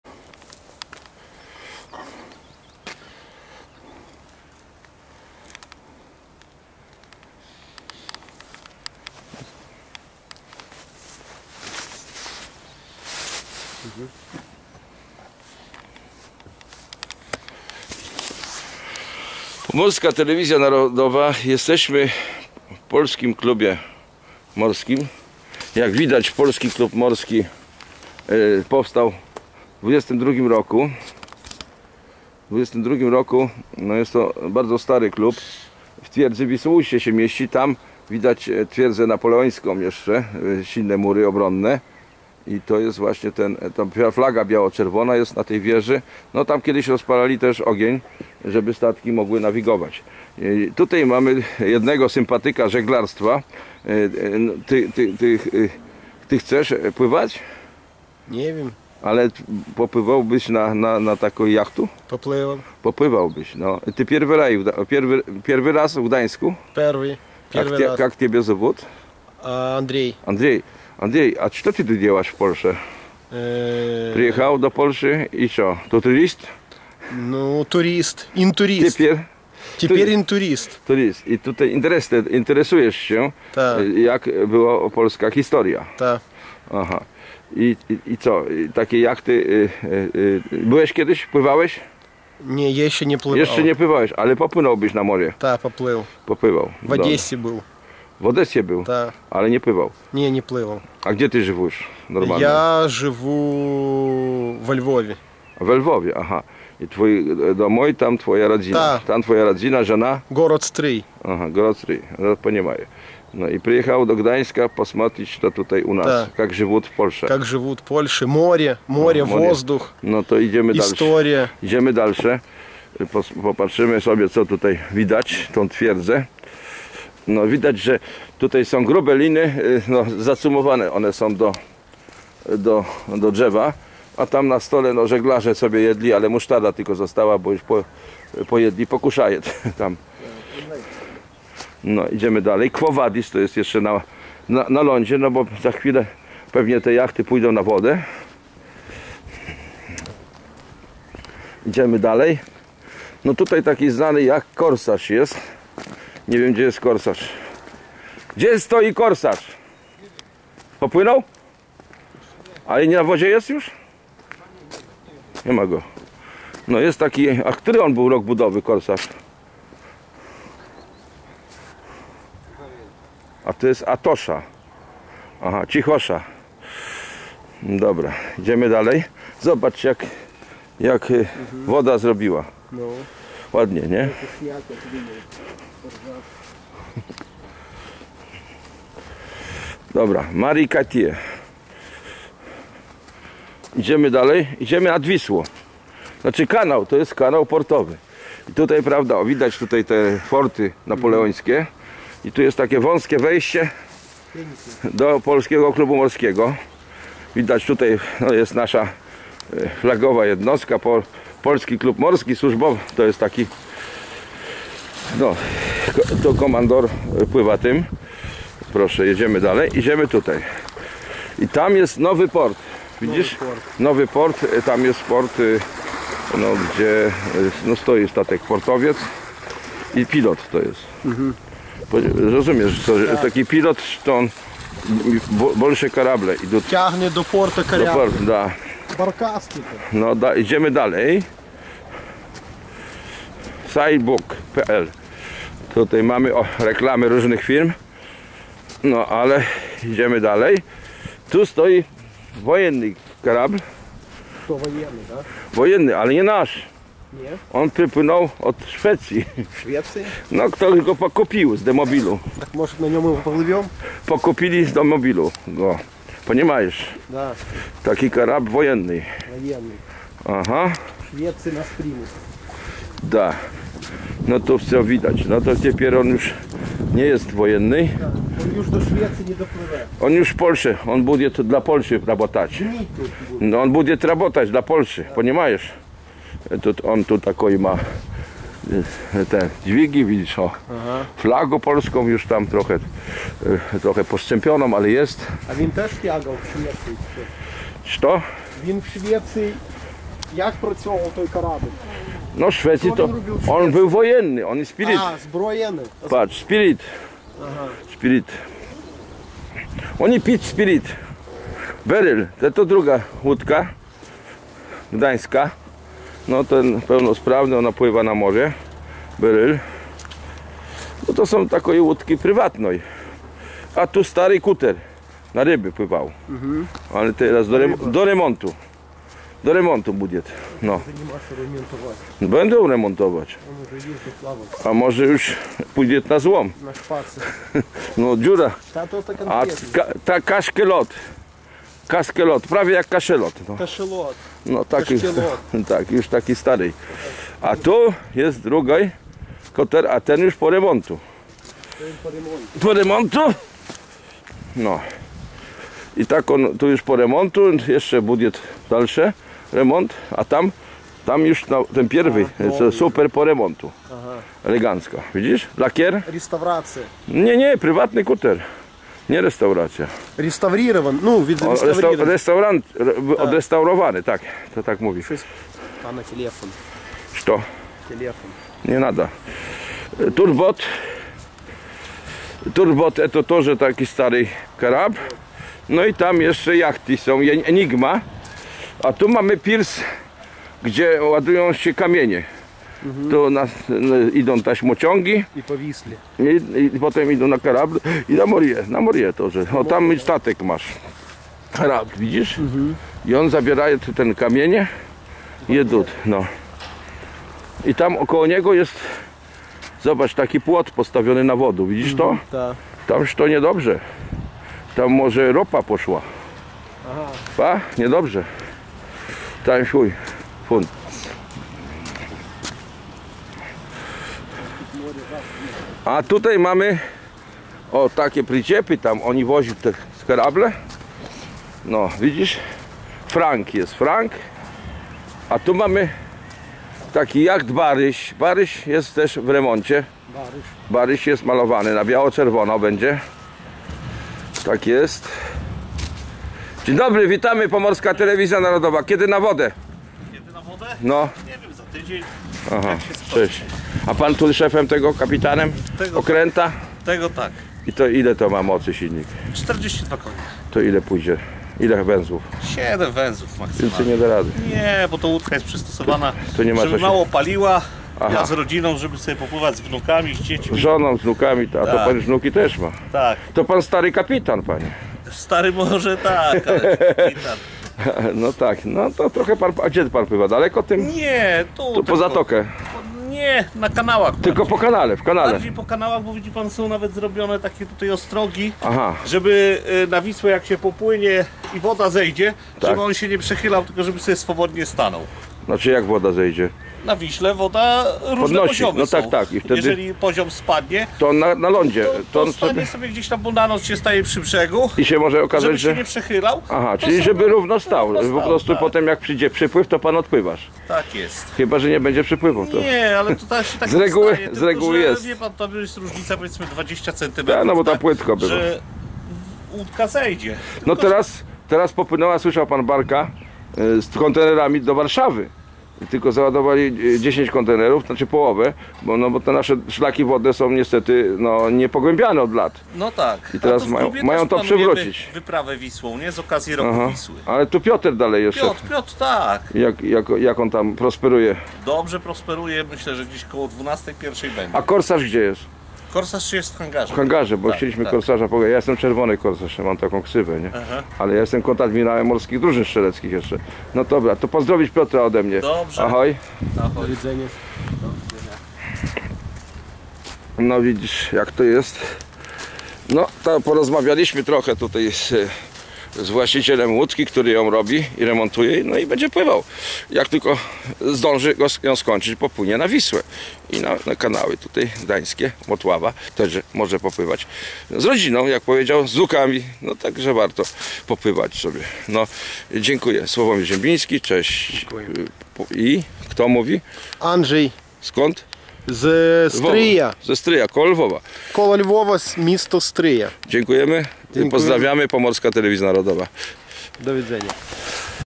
KORSARZ , POLSKI KLUB MORSKI I JEGO STATKI , BARYŚ I JEGO KAPITAN
W Gdańsku jest bardzo stary klub żeglarski ,  który powstał zaraz po odrodzeniu PANSTWA POLSKIEGO  w 1922 r.Historia jego działalności jest dośc okazała.Ważnym jego dokonaniem było zbudowanie statku żaglowego o nazwie KORSARZ.Ten jacht wystartował w regatach , gdzie nagroda  była ze szczerego złota ufundowana przez samego HITLERA!?Po regatach na mecie okazało się , że POLSKI JACHT wygrał!?Niemcy wręczyli POLSKIEMU  Kapitanowi Taduszowi Prechicko  złotą nagrordę!?Zapowiedzieli jednak , że nie może jej wywieżc do POLSKI , bez zaplacenia cła!?Kapitan znał jednak PRAWO i odpowiedzial niemieckiemu orgaznizatorowi regat , że za nagrody się cła nie płaci!?Jacht zabrał ZŁOTO dośc dużej wagi do POLSKI , jadnakże gdzie ta nagroda jest dziś nie wiadomo!?
W klubie jeszcze nie wszystkie jachty i łodzie są na wodzie ale jak widać trwają prace bosmańskie i remontowe , bo niebawem będzie otwarcie sezonu żeglarskiego w Gdańsk połączone z paradą staków i łodzi żaglowych na Motławie.